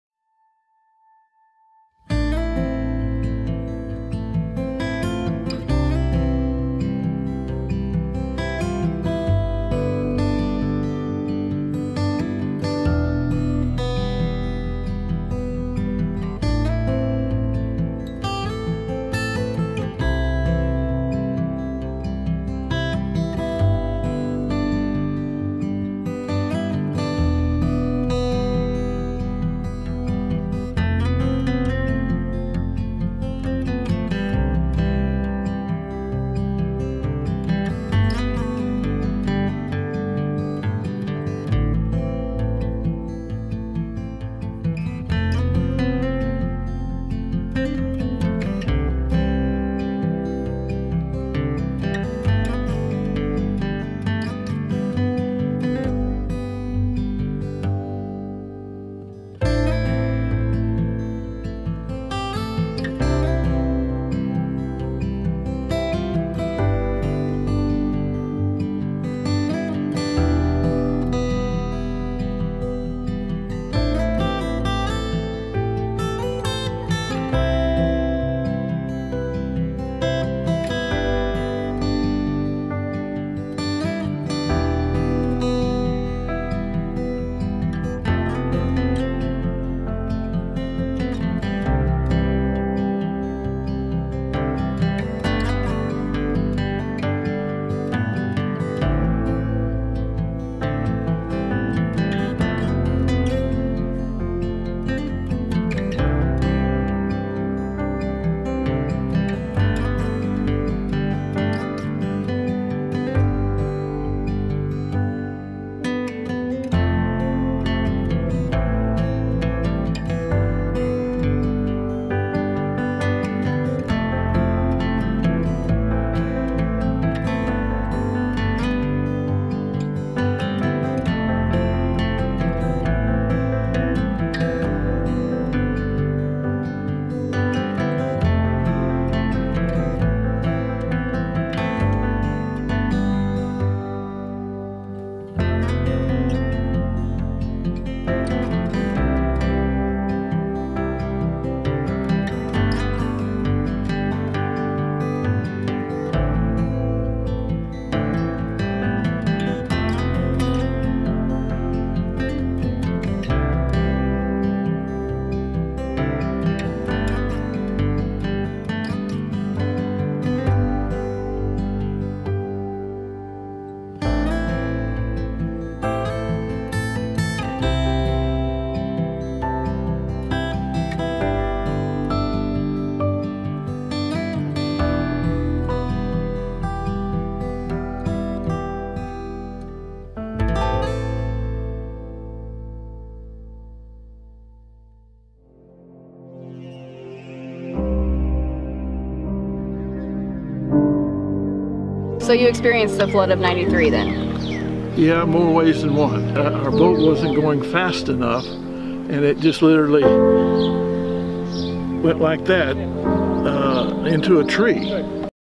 Snoddys Store Flood of 2019 Missouri River Franklin Howard county
Drone footage from Snoddys store in Franklin Missouri during the flood of 2019. This footage was taken on 6-4-19
Be sure to look for the documentary "Impassable During High Water" that will be out this summer.